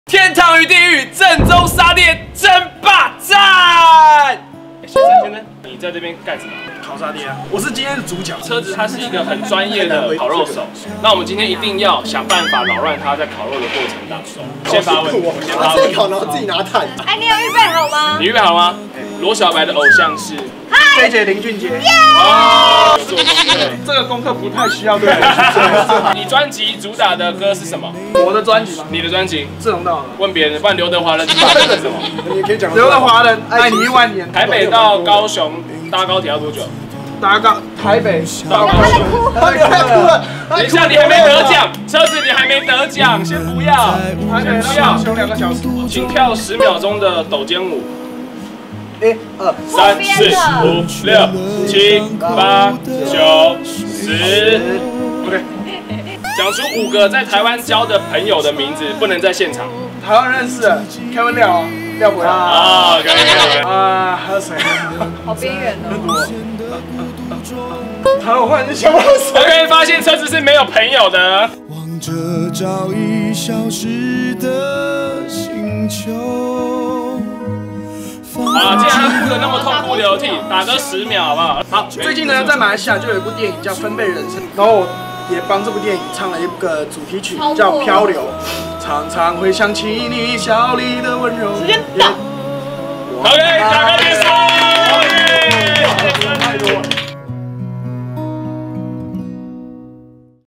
WebTVAsia 正宗沙嗲爭霸戰【車志立-車子】挑戰快問快答 竟直接飆淚？！
車志立挑戰快問快答，居然直接飆淚？！

烤肉趴嘉賓：羅小白S.white、黃美珍、Haor 許書豪、車志立-車子、Gino勁宥、100種理想、羅文裕 WING

上行娛樂 X Webtvasia Taiwan 特輯